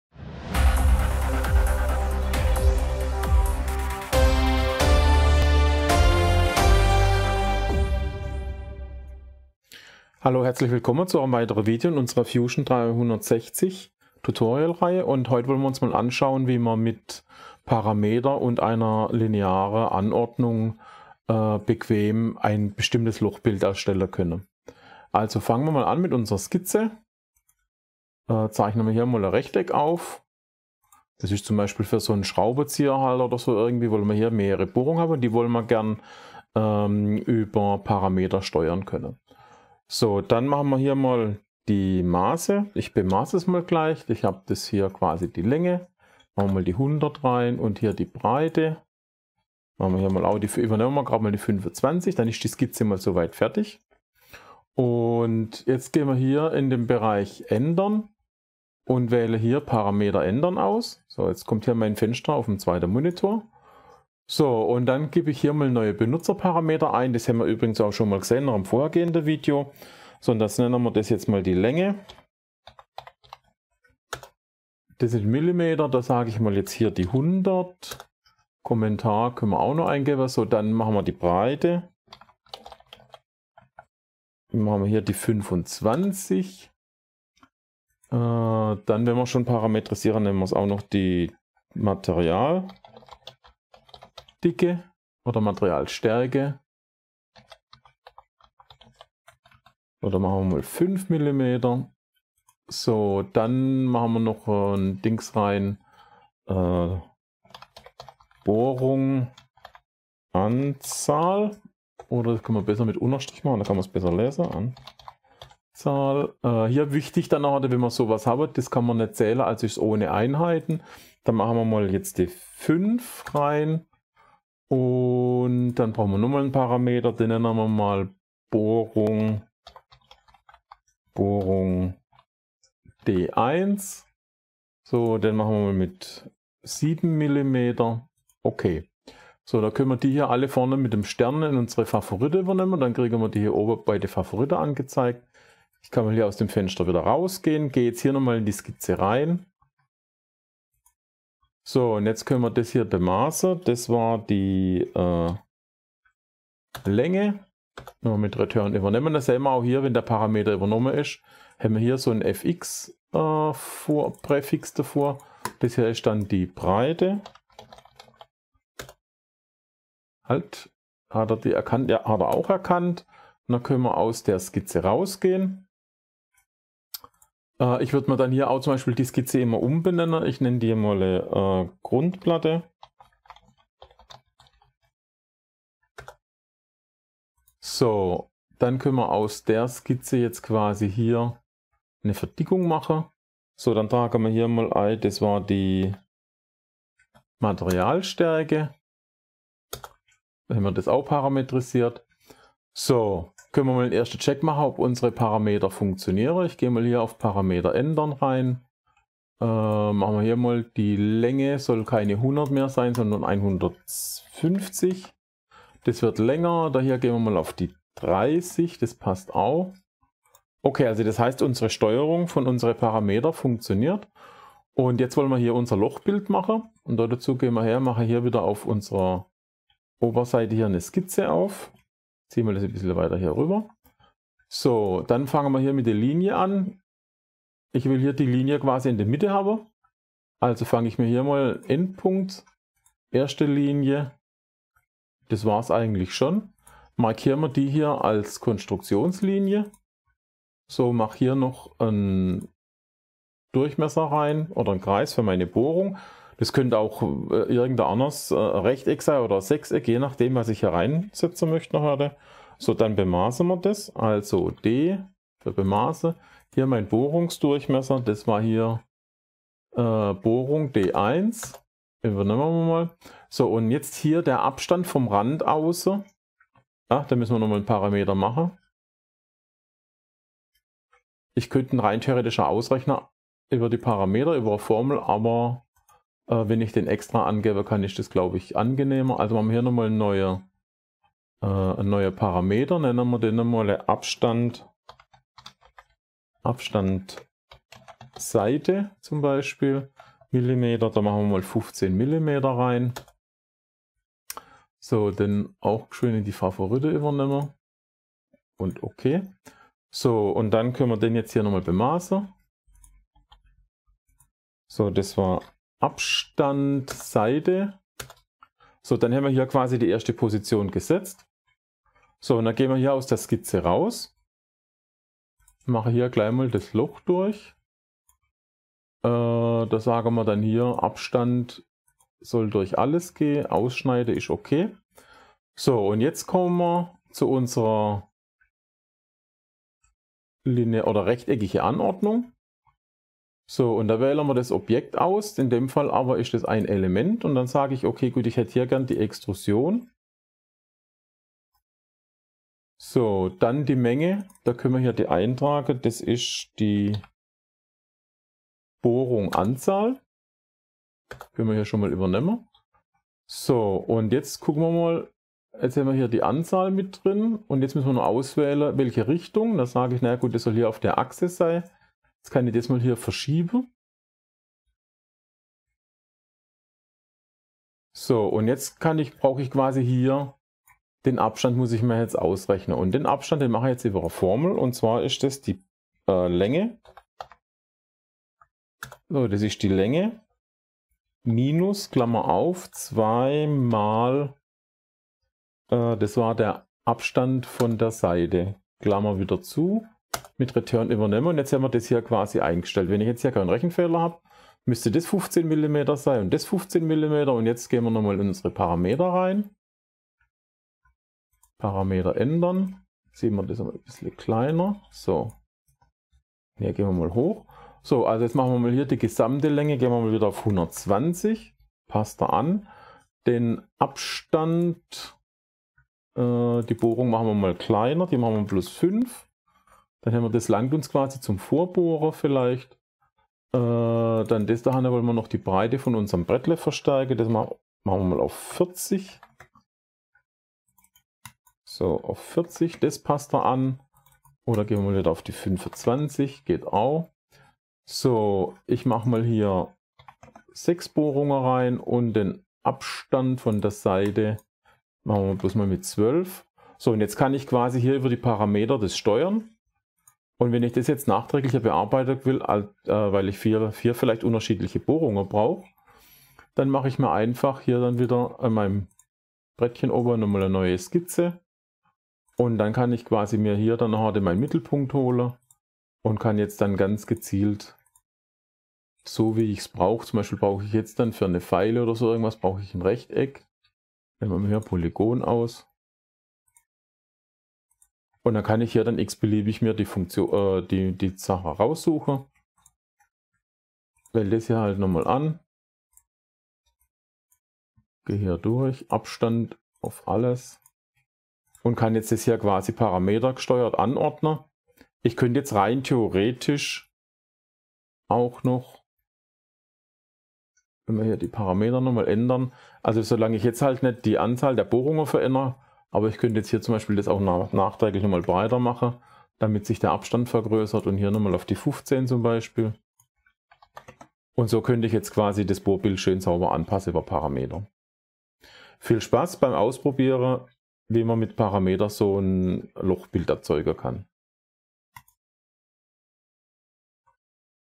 Anordnung mit Parameter in Fusion 360 - Rechteckige Anordnung mit Parametern konfigurieren
Anordnung mit Parameter in Fusion 360 versehen und dann bestimmte Maße und Abhängigkeiten mit Formeln berechnen ist einfacher als man denkt :-). In diesem Video sehen wir, wie man in ein Rechteck eine Anordnung von Bohrungen mittels Parametern steuern kann.